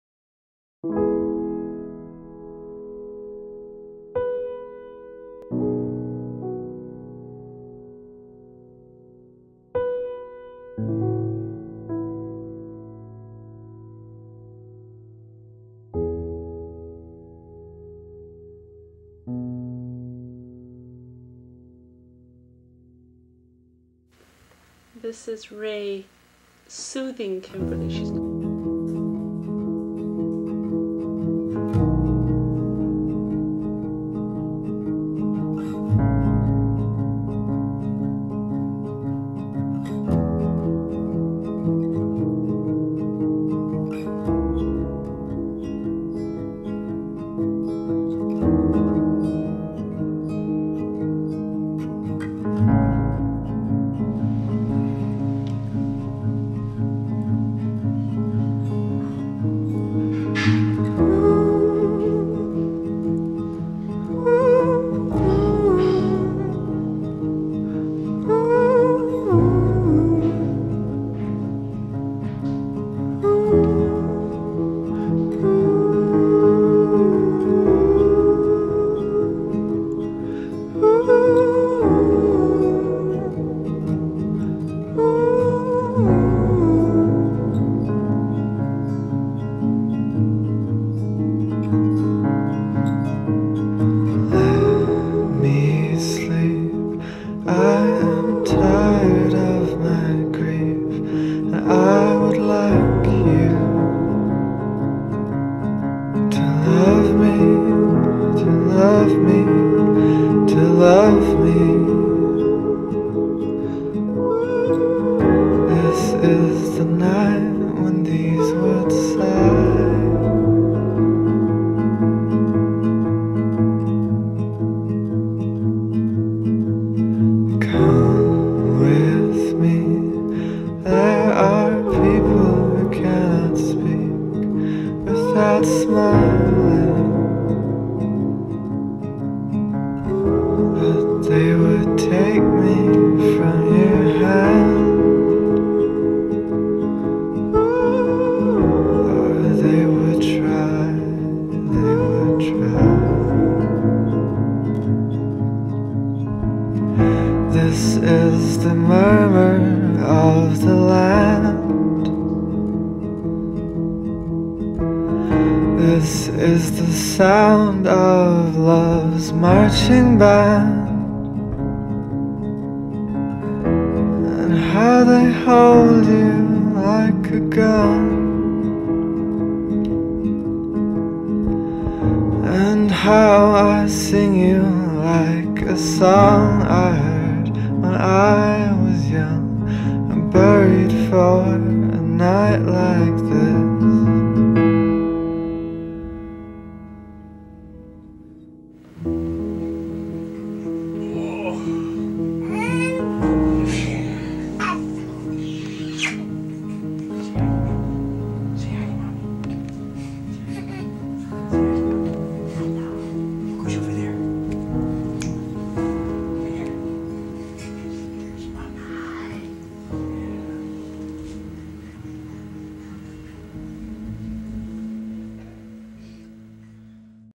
I Am Tired Of My Grief
Today is my dad's birthday.  He would have been 59.  I have not healed.  I don't think I'll ever really heal.

I would give anything to have one more day with him, to tell him how much I love him and miss him.